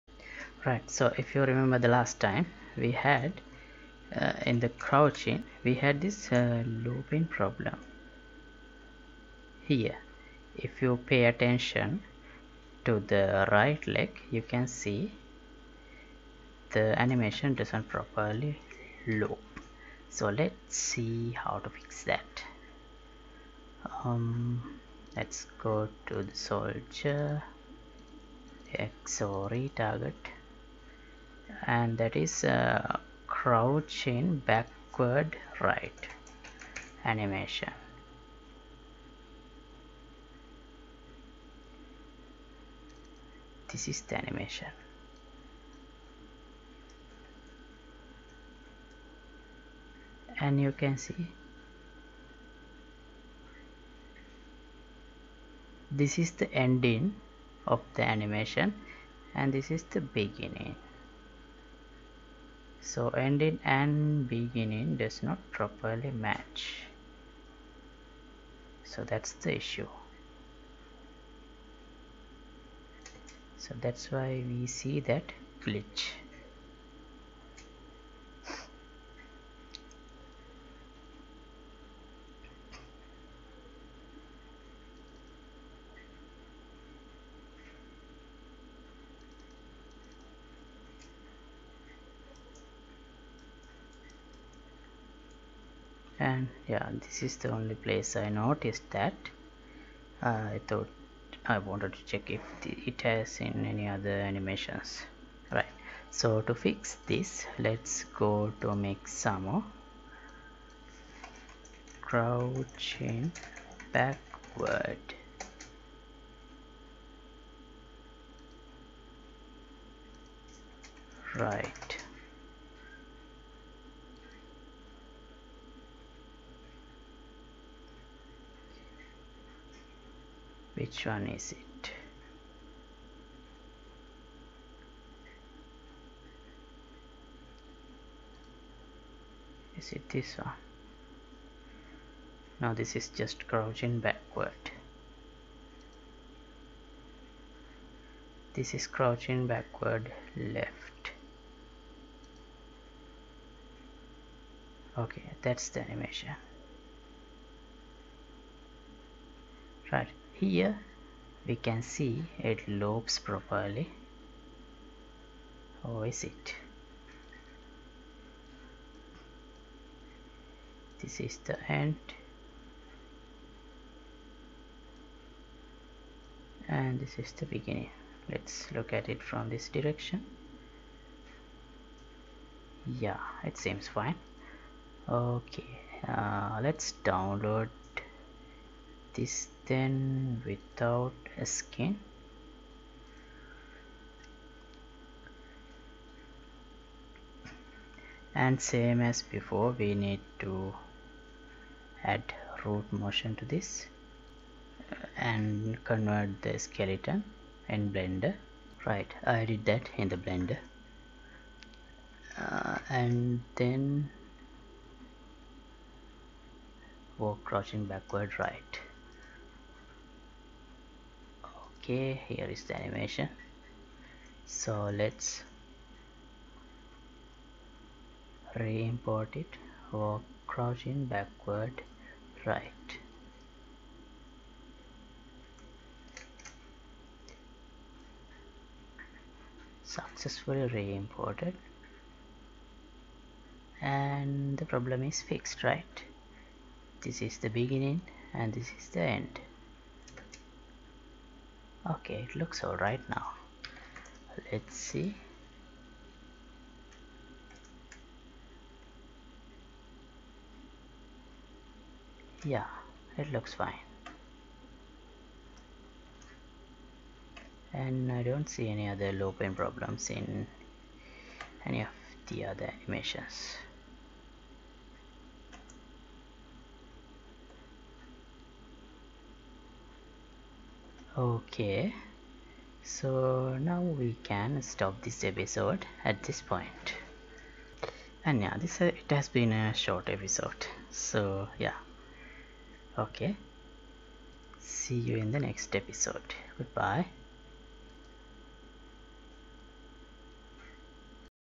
Unreal Third person Shooter #56 - Animation Loop Matching
In this episode of my unreal engine third person shooter series, I am going to fix some animation loop mismatch problems I had in some animation I have used in the movement blendspace. I am going to show you how to fix that.